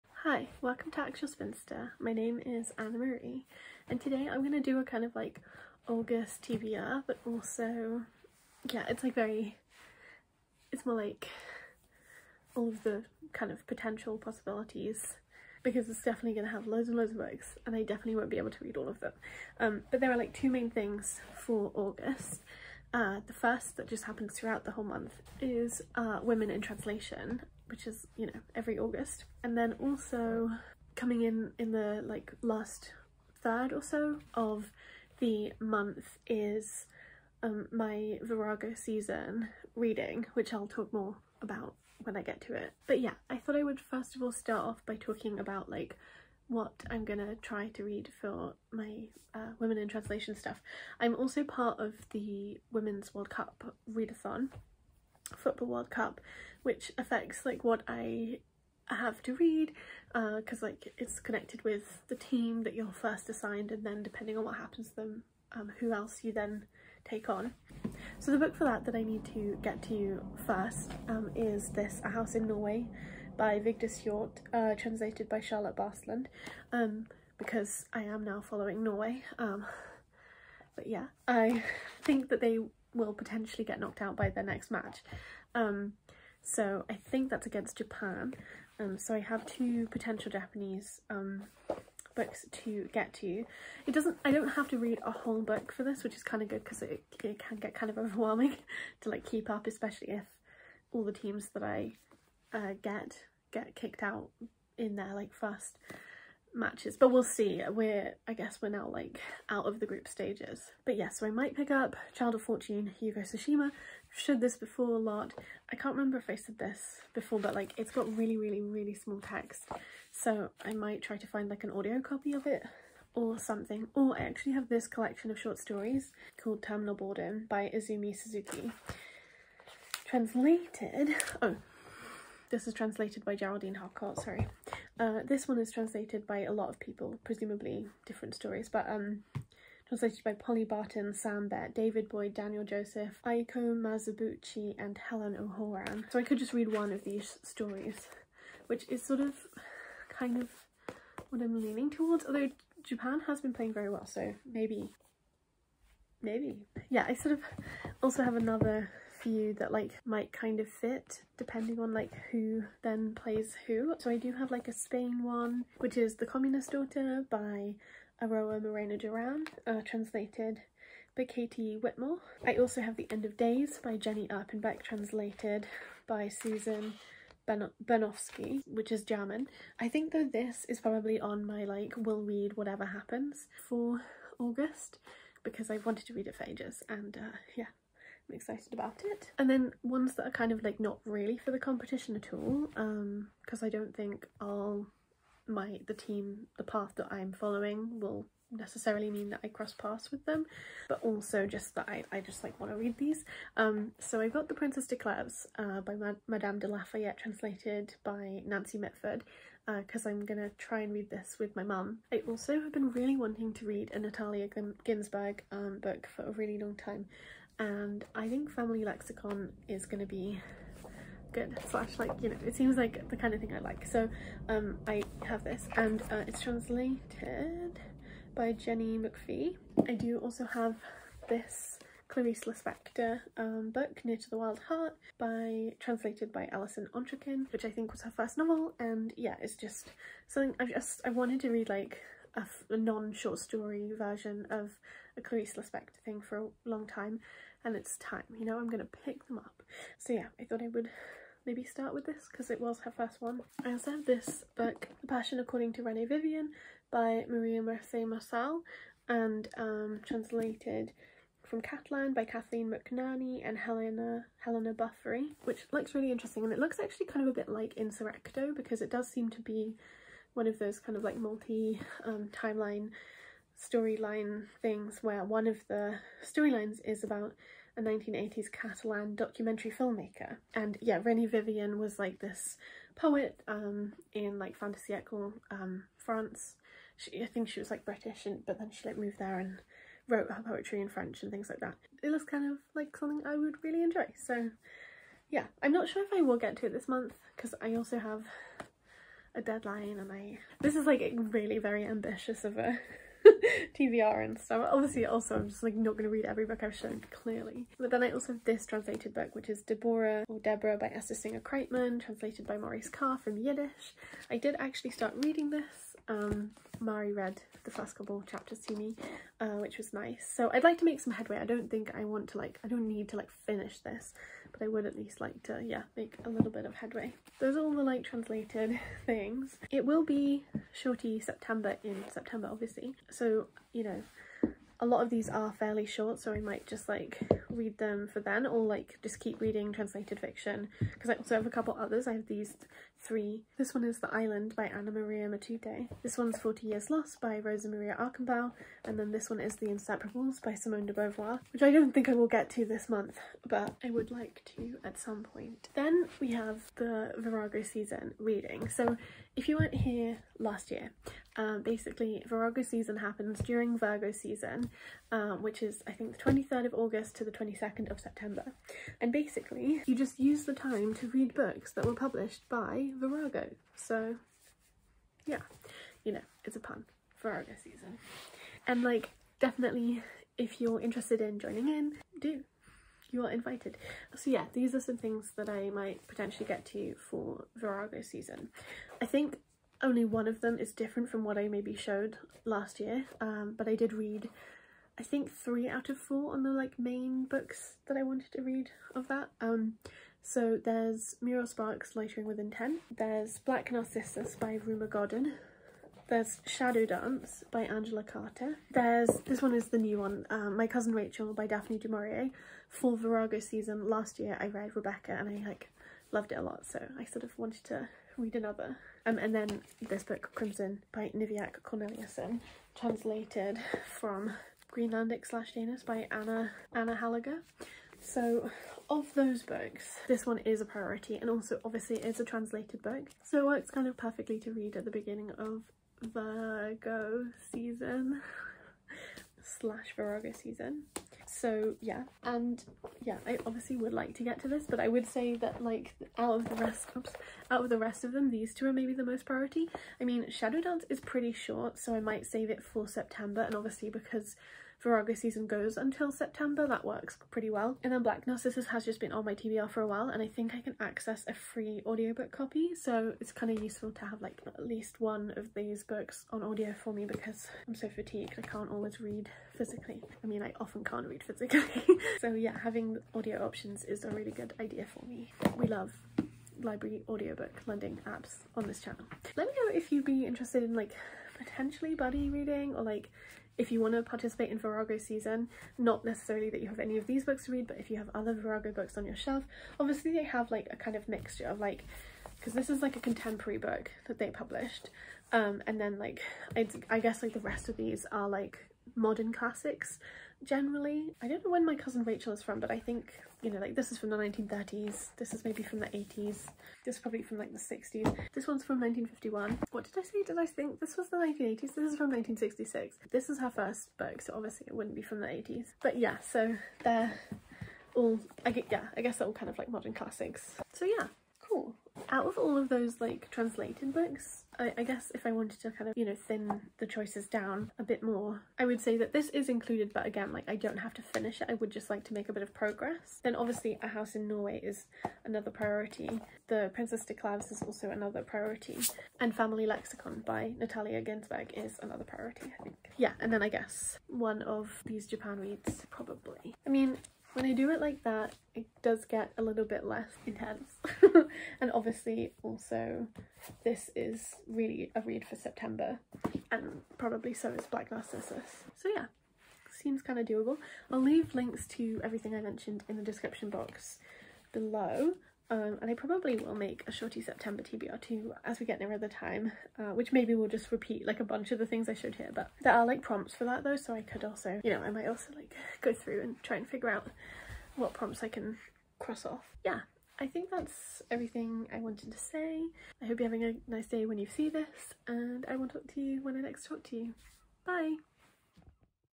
✨✒️🍎 august tbr | women in translation month & virago season 🍎✨✒️
things mentioned :
i have packed up my copy of apology for the woman writing by jenny diski but i would love to get to that during virgo season too. but it is probably unlikely !!